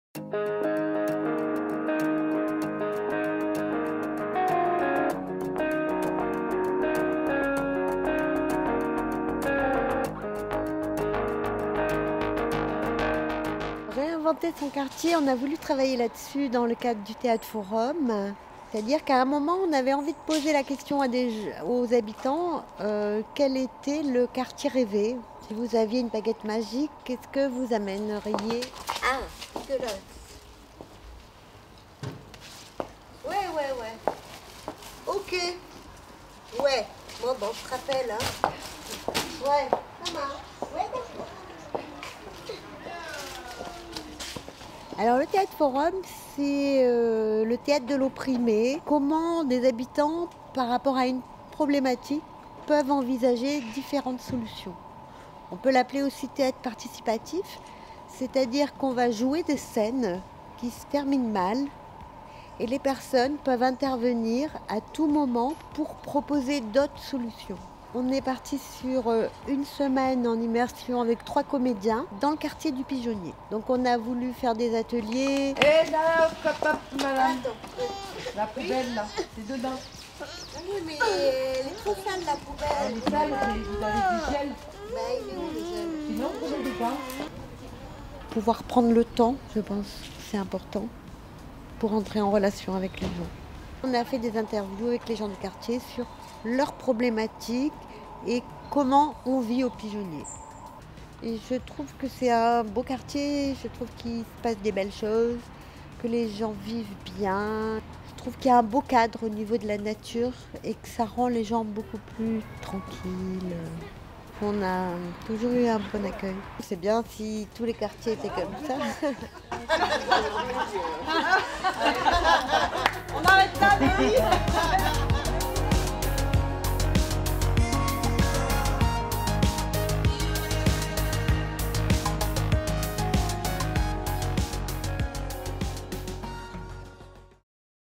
Réinventer son quartier par la compagnie GOBAN 🎭
🎭 La compagnie GOBAN a passé une semaine au coeur du pigeonnier pour recueillir des problématiques que rencontrent les habitants du quartier. 💬

À travers une représentation sous forme de théâtre forum, les habitants ont pu intervenir sur les différentes scènes et ont pu proposer des solutions. 📢

🎥 Tema TV a filmé la représentation pour vous. 👏👏👏